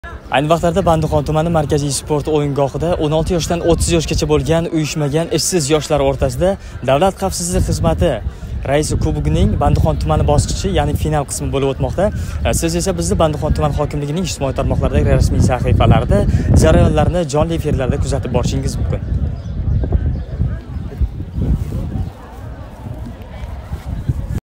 ДХХ раиси кубоги учун бўлиб ўтган спорт мусобақалари ғолибларини тақдирлаш маросими
ДХХ раиси кубоги учун бўлиб ўтган спорт мусобақалари ғолибларини тақдирлаш маросими ҳамда концерт дастури қисқа фурсатлардан сўнг бошланади.
Барчангизни таклиф этамиз

📺 ʏᴏᴜᴛᴜʙᴇ ➣ ᴛᴇʟᴇɢʀᴀᴍ |
📸 ɪɴsᴛᴀɢʀᴀᴍ ➫ FaceBook |